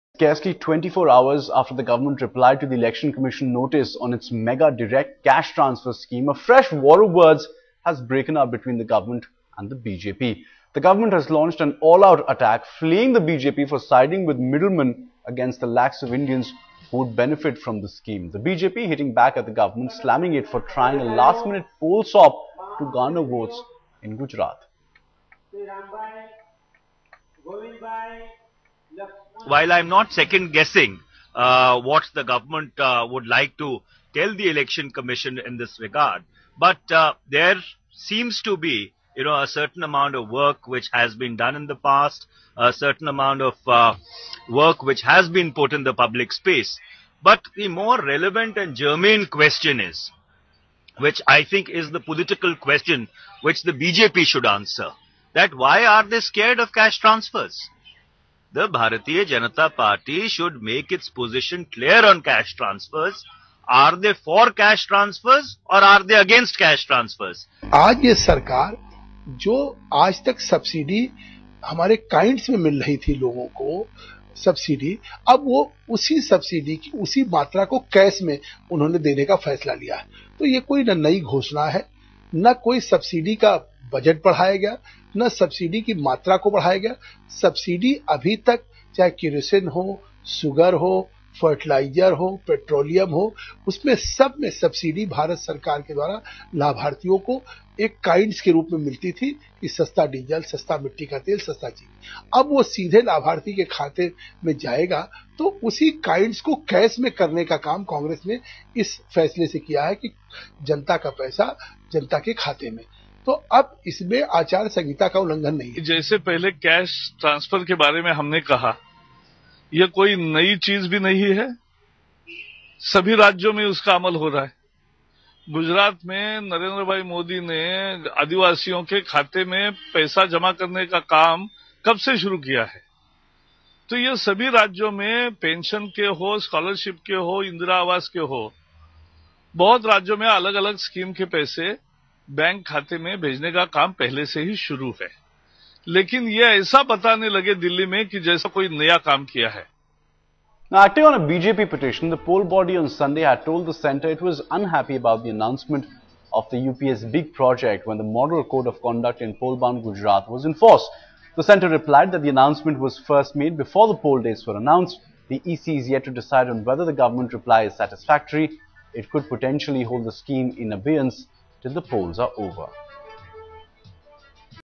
War of words over cash transfer, BJP asks what's new in the scheme
BJP leaders insist that the latest dole out scheme is a no brainer. Similar schemes in the form of pension and scholarship are already in force